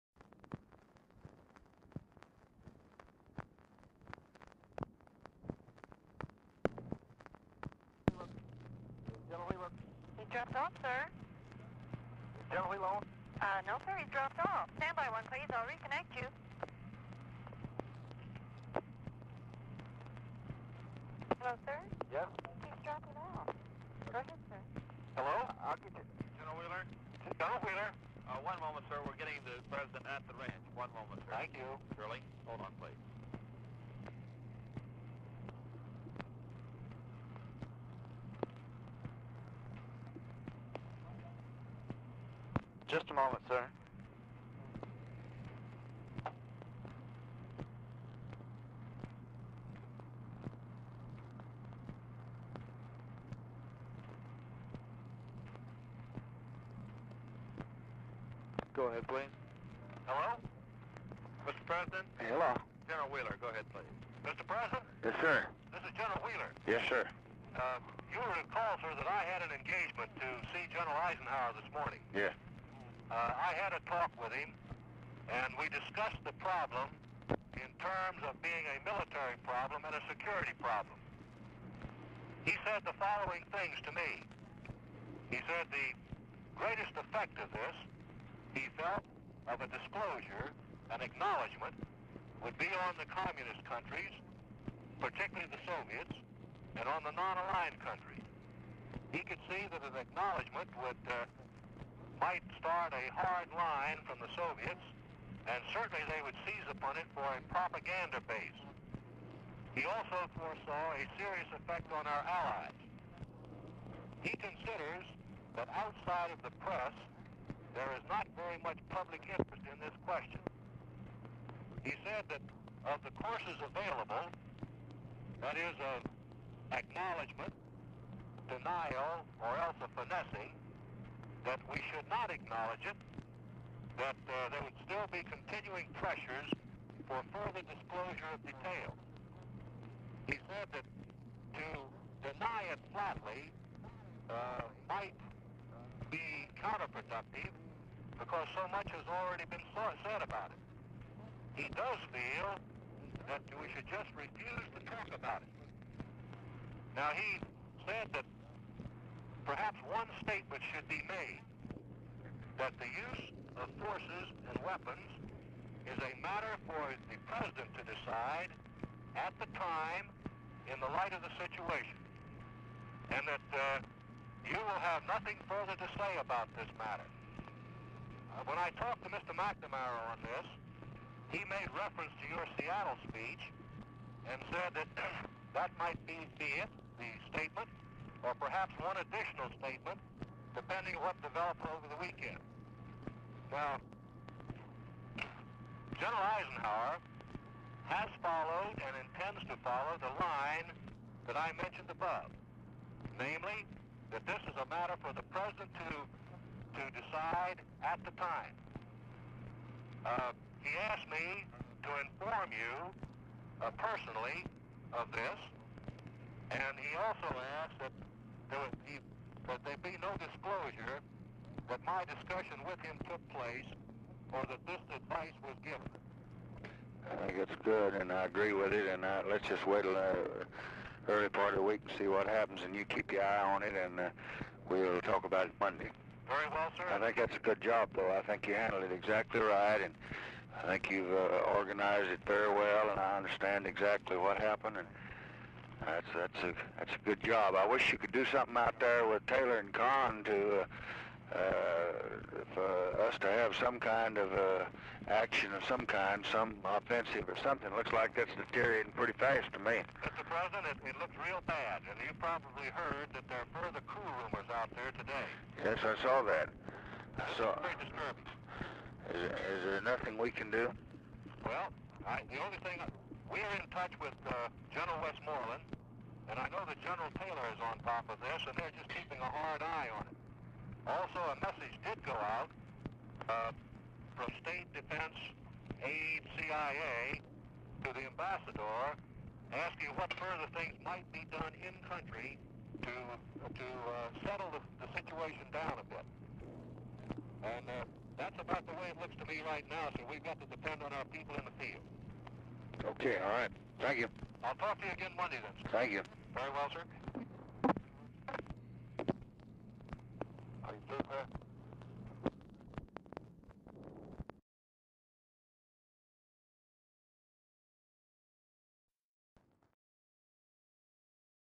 LBJ and Earle Wheeler, 9/26/64, 11.52a.
Telephone Conversation between President Johnson and Earle Wheeler.

Time: 11:52AM

Location: LBJ Ranch
Speakers: President Johnson and Earle Wheeler

General Topics: Defense; Elections; National Politics; Presidency; Vietnam

Topics: Wheeler Reports On Meeting With Eisenhower About Delegation Of Authority To Use Nuclear Weapons; Eisenhower's Recommendations That Detailed Disclosure Of Authority Not Be Made And His Meeting With Wheeler Not Be Reported; Rumors Of Coup In South Vietnam